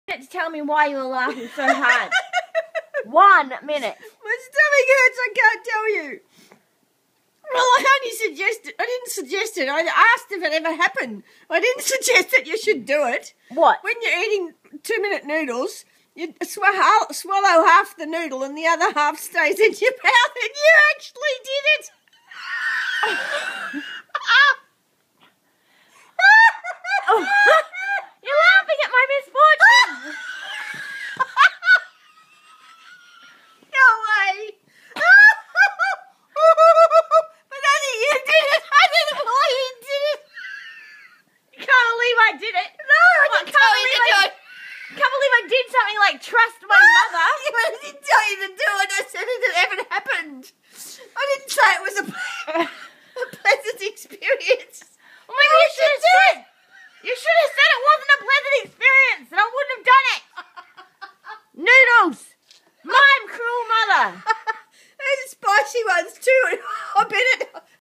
My 2 minute noodle mad mother
Mum crying with laughter after my noodle misfortune (which she suggested I do). Anti mother of the night award goes to this crack pot!!